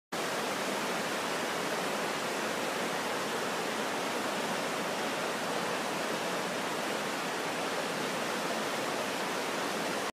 Uji River
The water flowing very high on the Uji river. In Uji city, Kyoto prefecture, Japan.